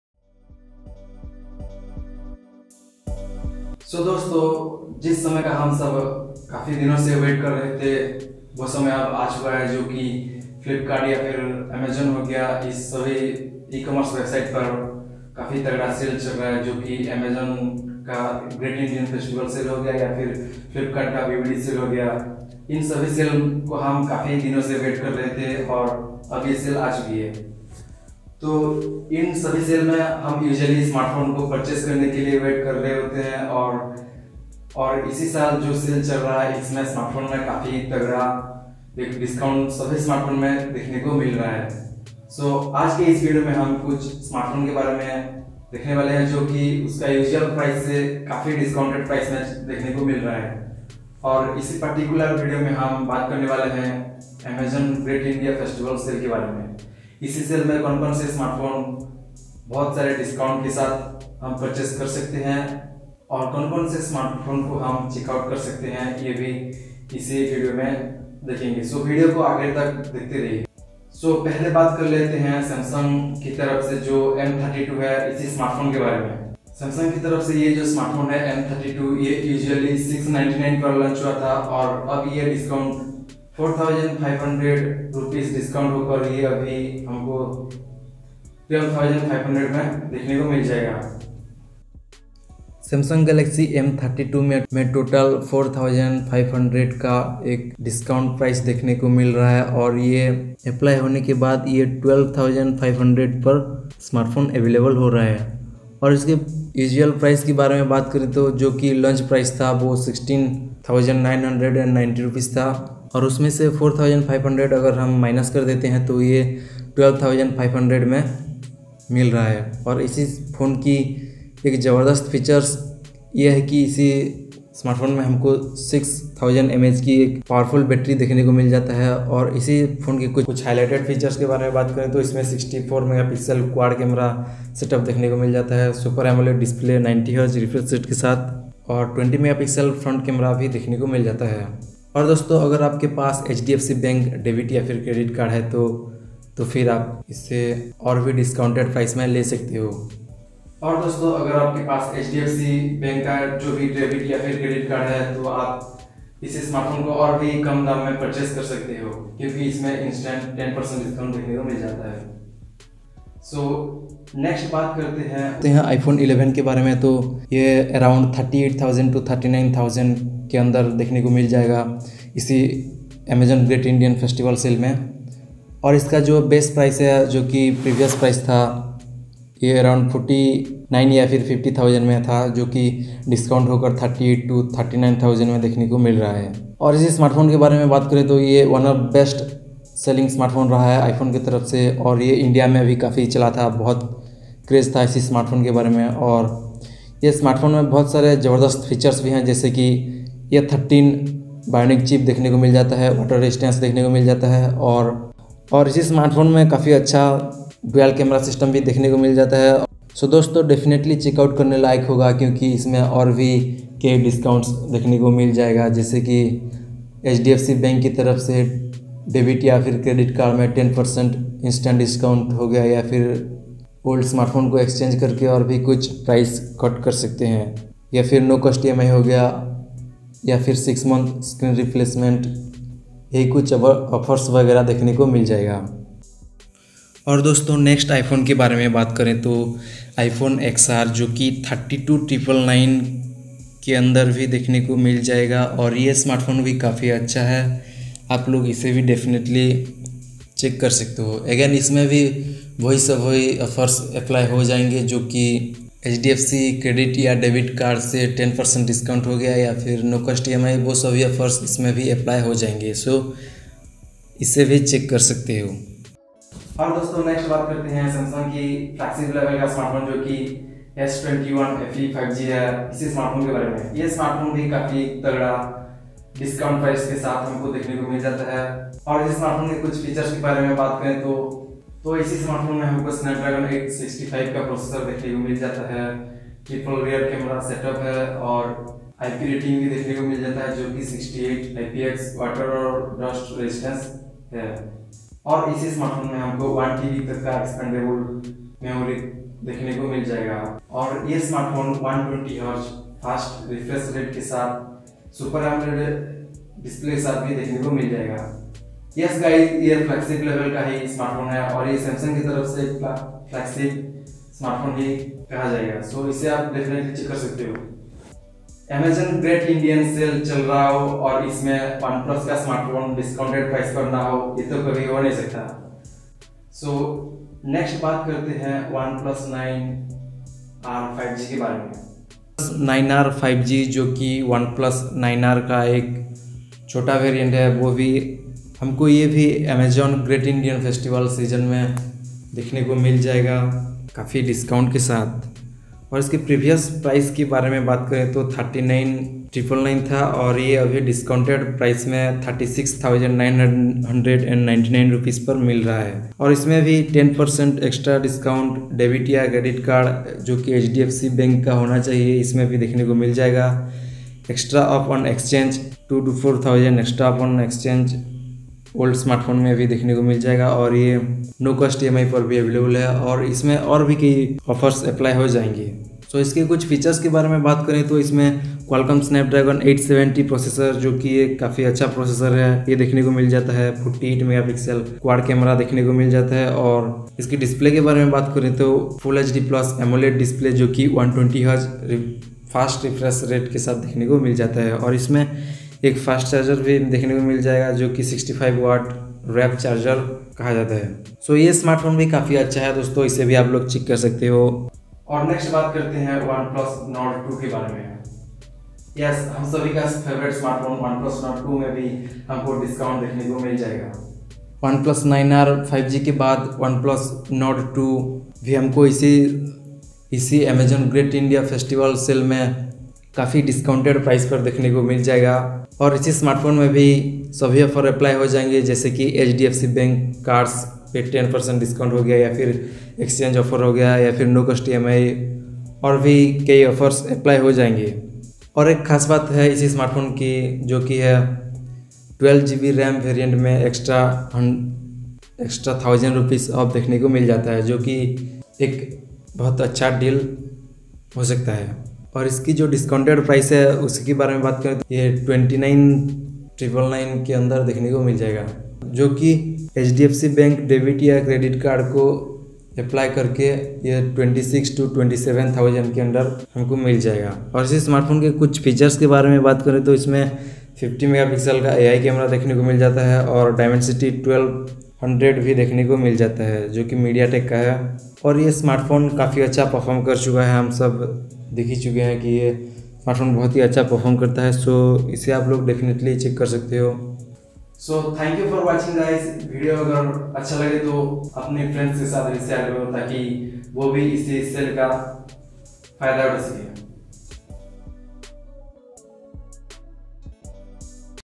Best Amazon Great Indian Festival 2021 Deals ⚡ Feat. Top Deals on Smartphones
Amazon and Flipkart Sale is Here and here are some biggest deals from them.
Amazon great Indian sale 2021 is currently live and in this video i am talking about these sale about best mobiles deals to choose from with huge discounts.